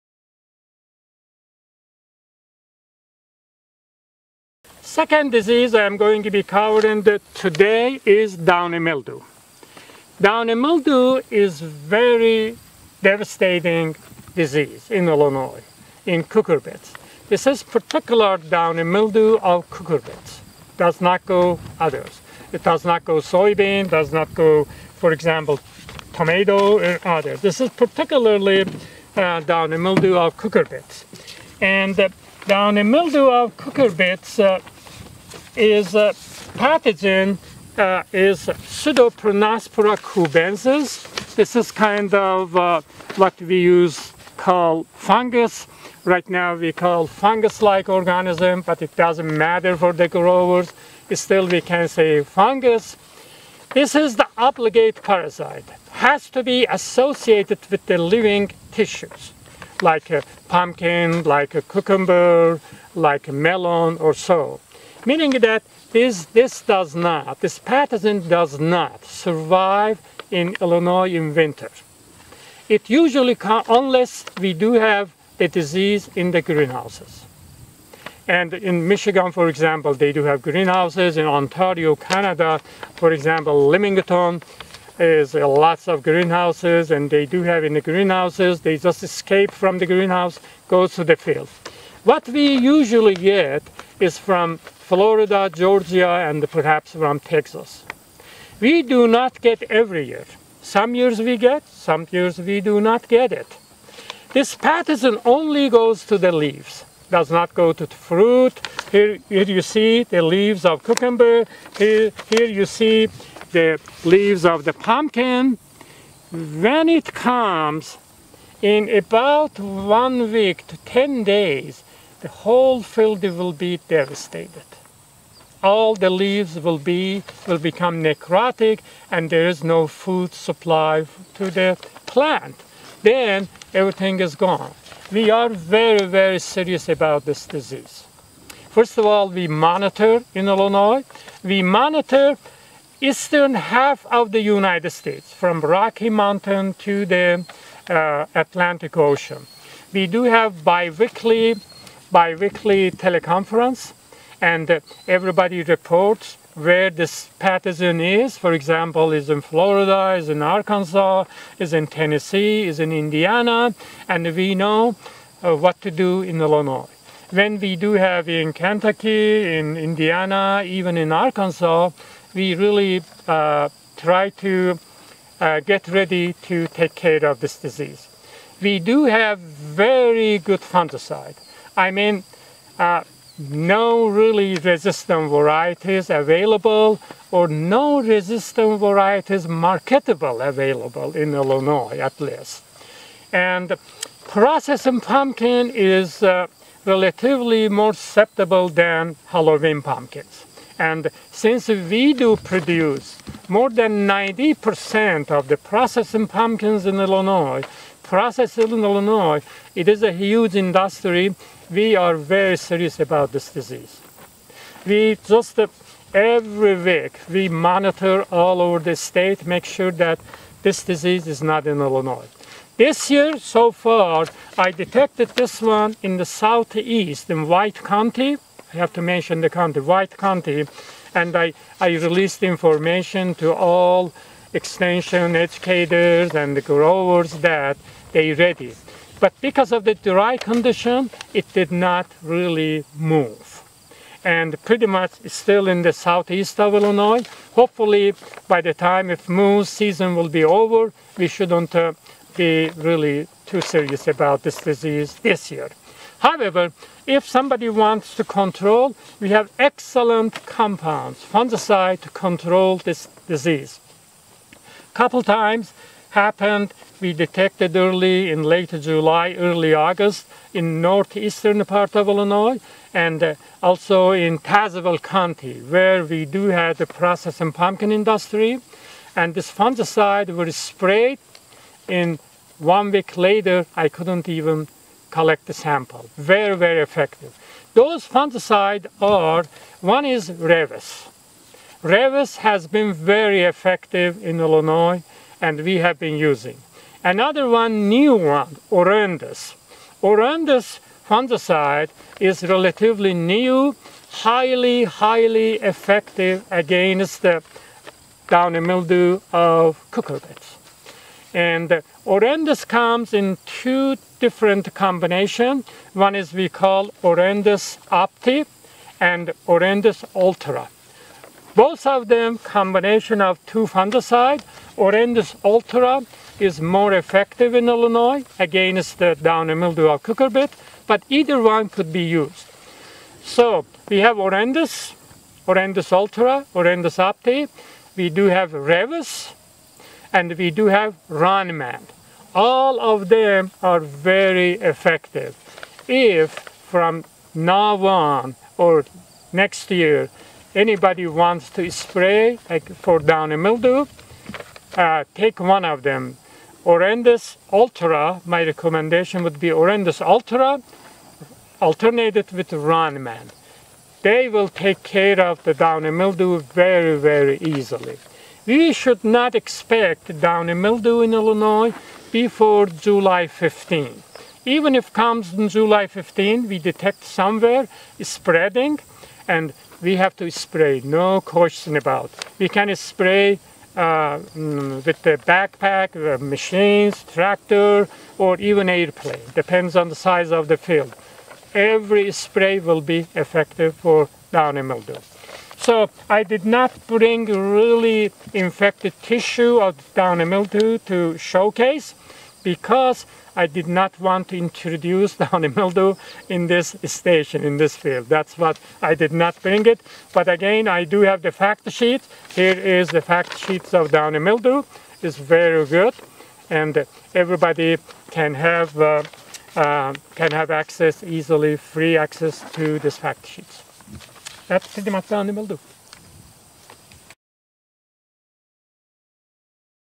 Downy Mildew Management in Pumpkins
University of Illinois Extension Plant Pathologist, Mohammad Babadoost talks about current best practices for downy mildew management in pumpkins.